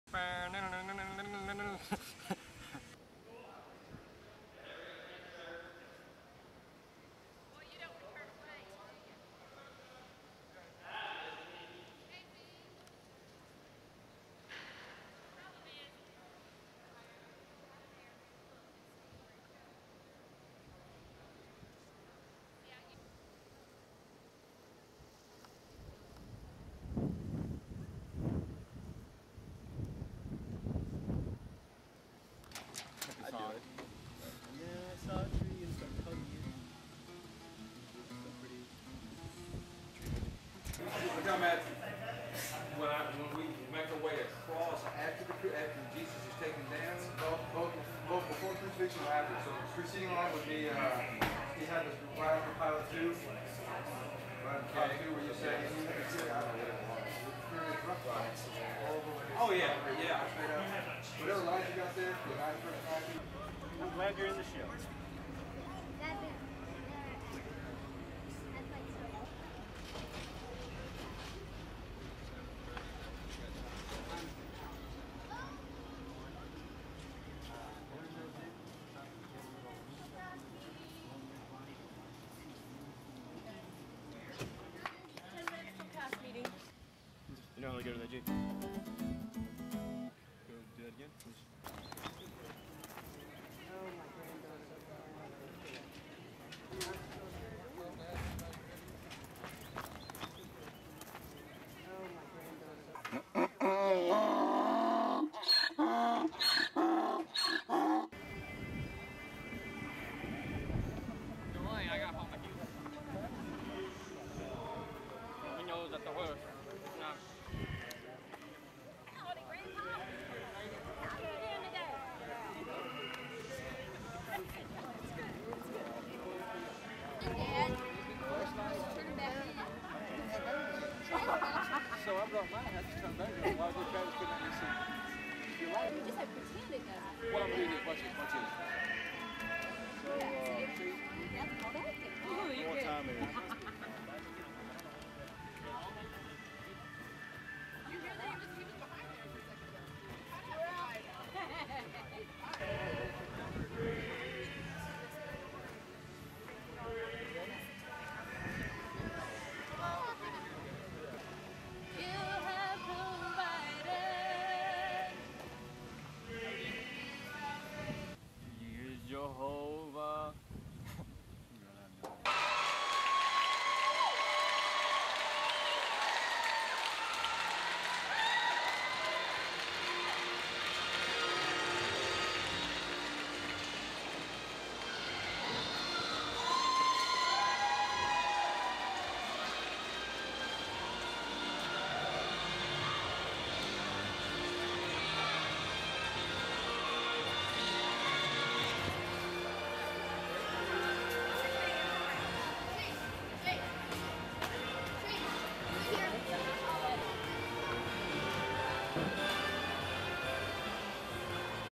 2006 The Promise Opening Night
The Promise is a theatrical passion play about the life of Jesus Christ acted on the Texas amphitheatre in Glen Rose Texas. The 2006 season lasted September through October with thousands in attendance.